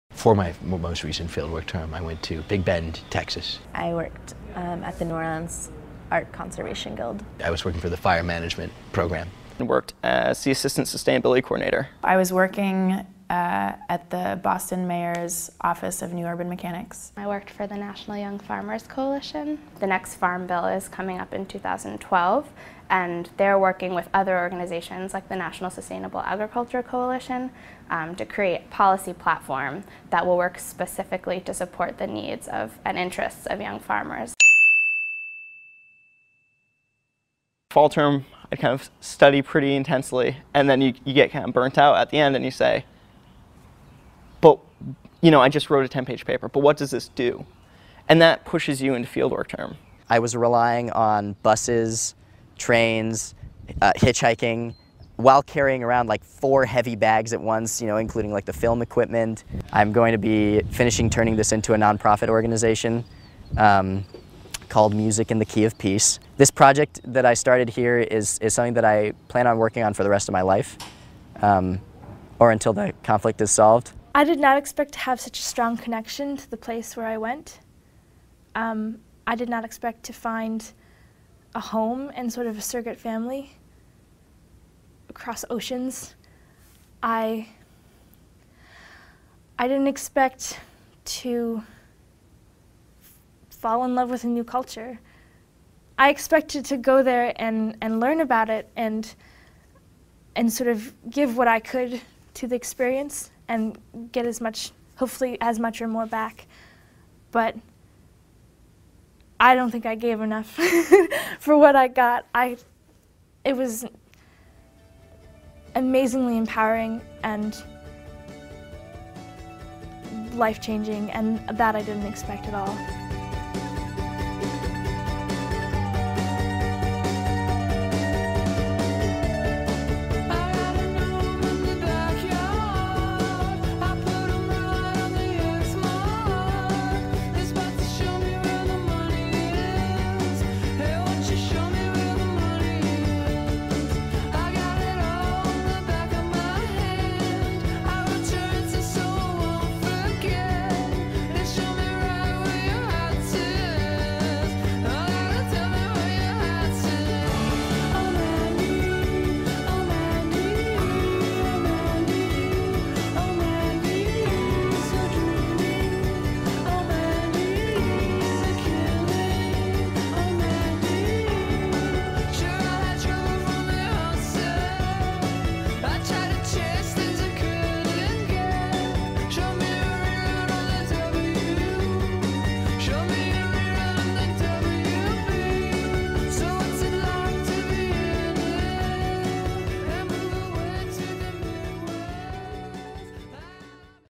Making Ripples in the World
Every winter our students bring their energy, insight, and ambition to the world for seven weeks during Field Work Term. Last year students volunteered more than 25,000 hours of time collectively helping to find shelter for homeless women and children; teaching ordinary citizens how to decode their own genetics; making green building models; going door-to-door for the EPA; and lobbying for healthcare and livable wages. Some have even created their own non-profit organizations. Every gift to Bennington is a gift to the world. Make your gift to The Bennington Fund today.